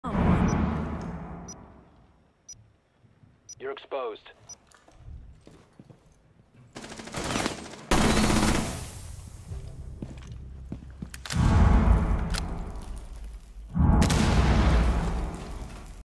Tricking Jackal!
Jackal thought he was smart. Used my clone to “hide” myself!